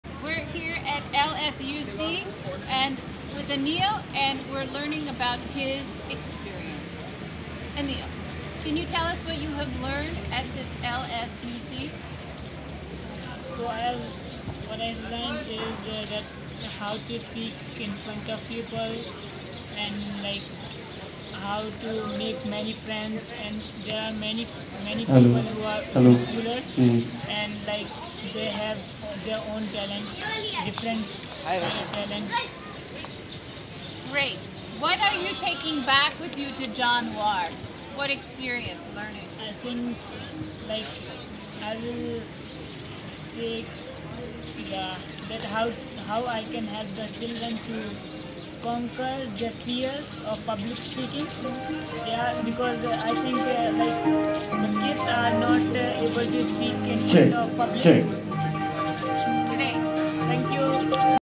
Take away from LSUC
Anil talking to Rachael Hammerlein at the Learning Societies Unconference Conference (LSUC).